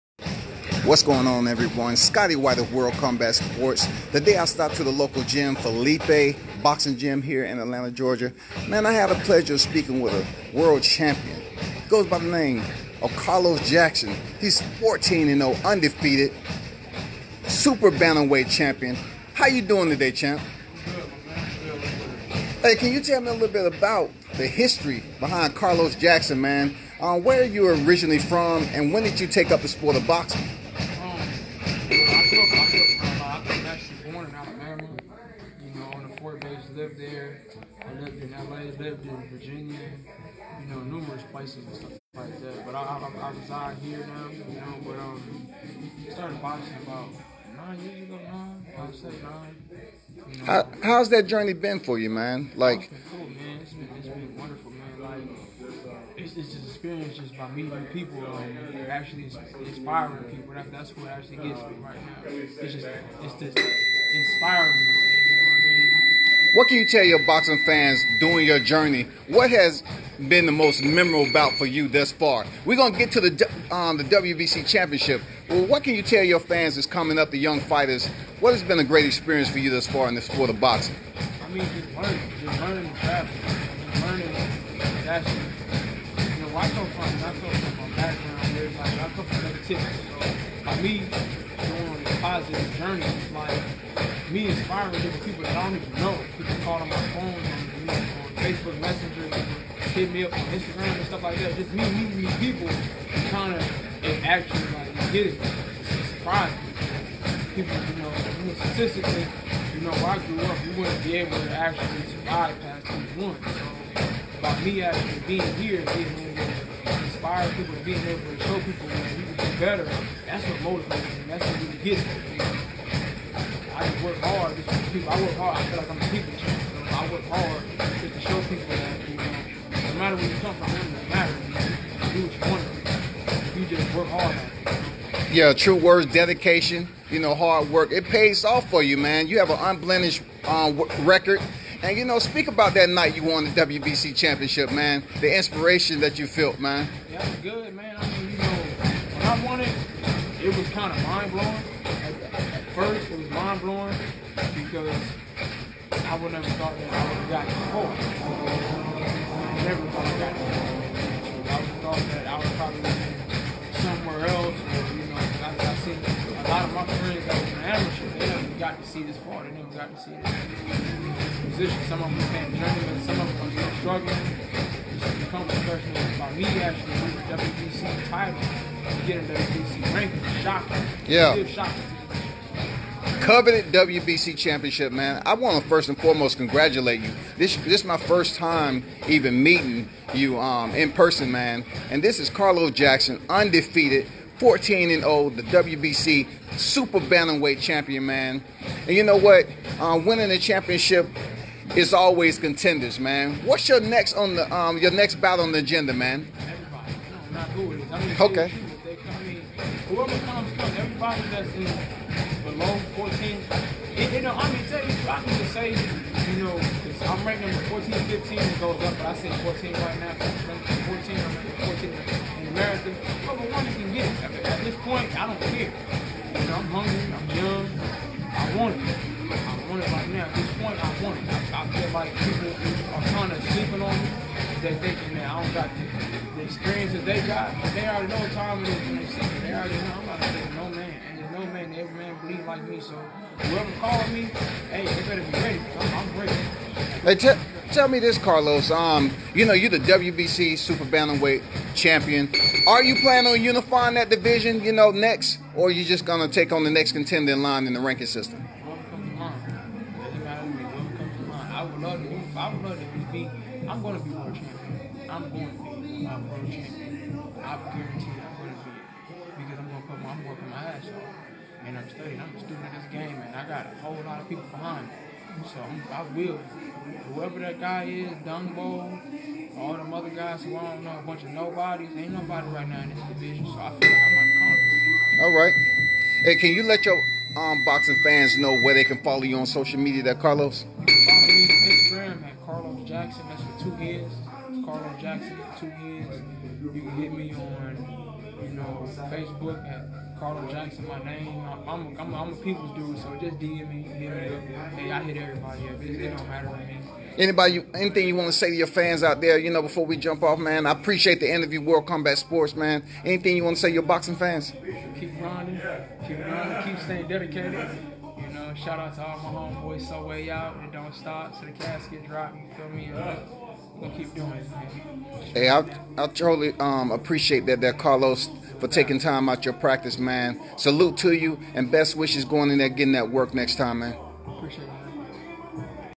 Professional Boxing Champion Carlos Jackson
Professional Super Bantamweight Champion Carlos Jackson speaks with World Combat Sports 5th December.